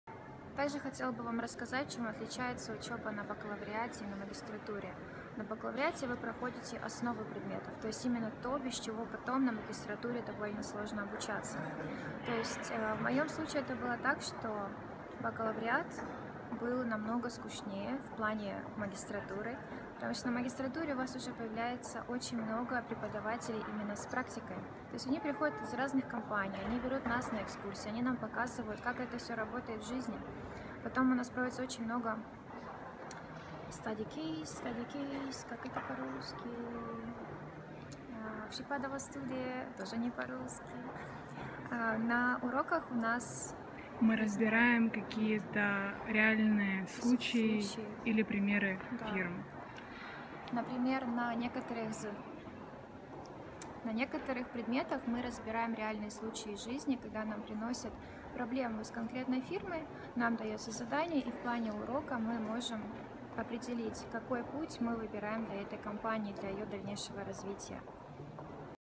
Бакалавриат и магистратура. Где учиться интереснее.
Студентка Пражского экономического университета Наталья продолжает знакомить нас со своей Альма-матер. Сегодня она делиться своими впечатлениями о том, как строиться обучение на  бакалавриате и в магистратуре. Где же интереснее? Давайте узнаем!